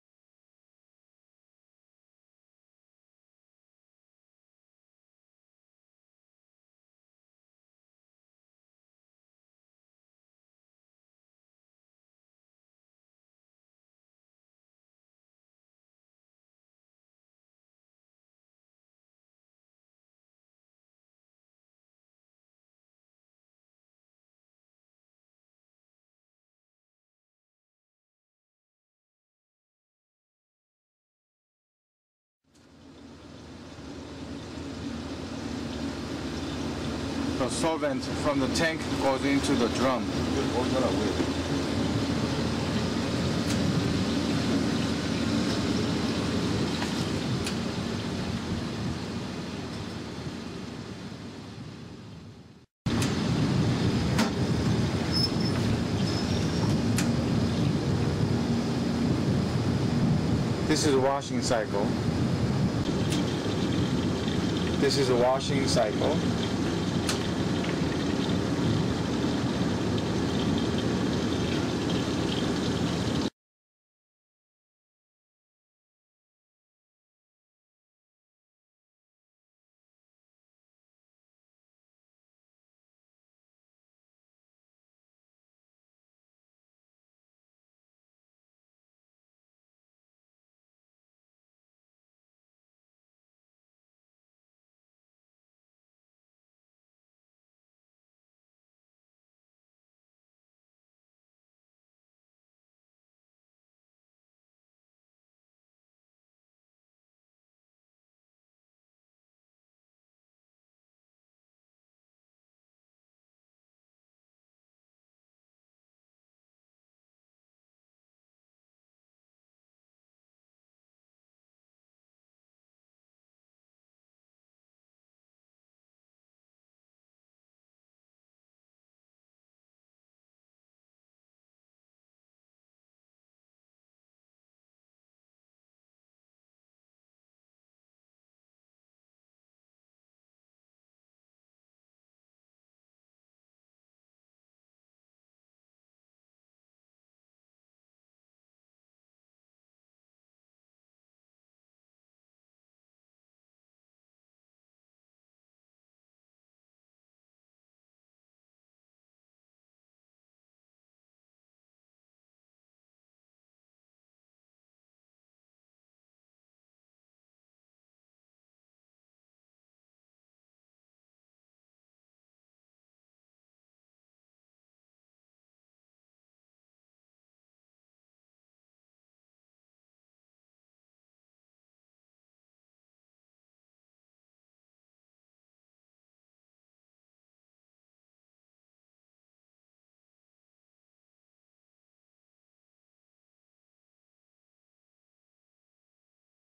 inconvenient truth of dry clean
This video tell you truth of how dry cleaning is done whether organic or regular